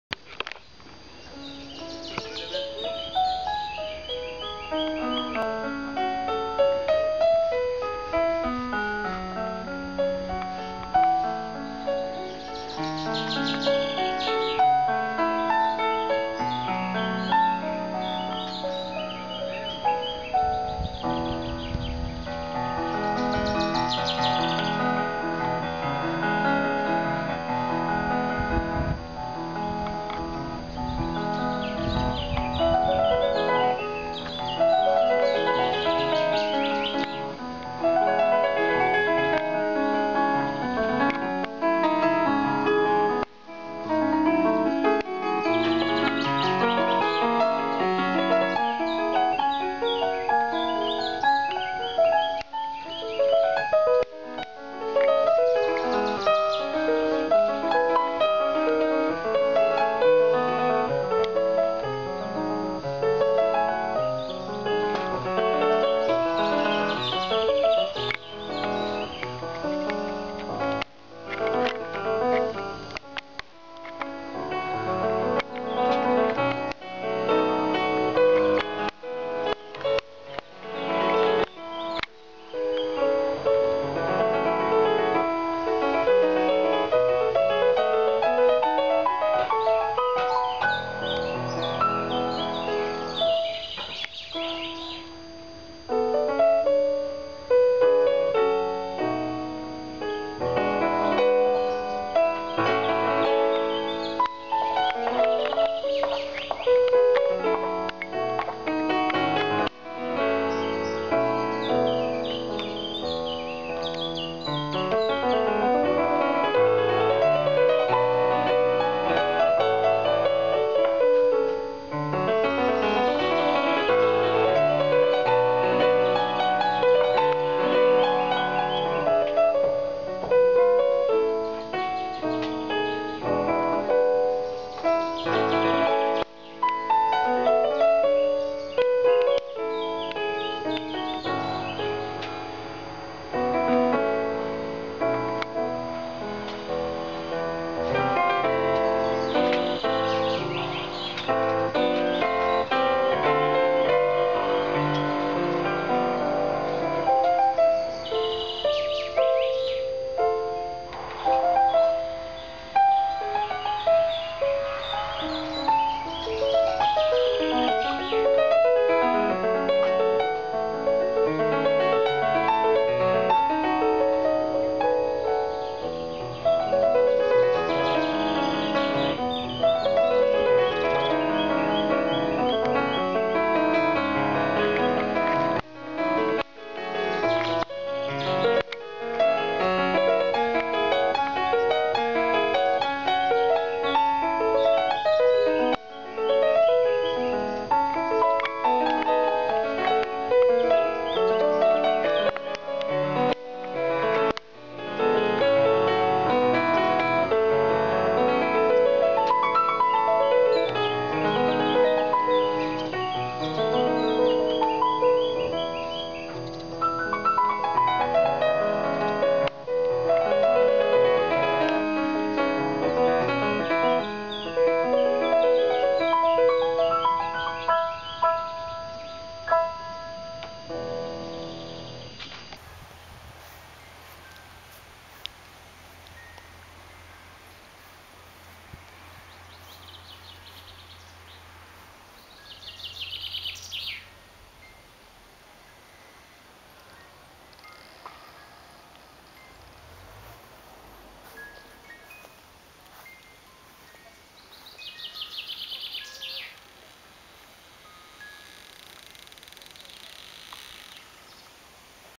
Avant concert vernissage exposition Chantier libre à Cercier
Arabesques matinales et bucoliques, prélude au concert "Musique à la belle étoile" du vernissage de l'exposition "Chantier libre" à Cercier 74350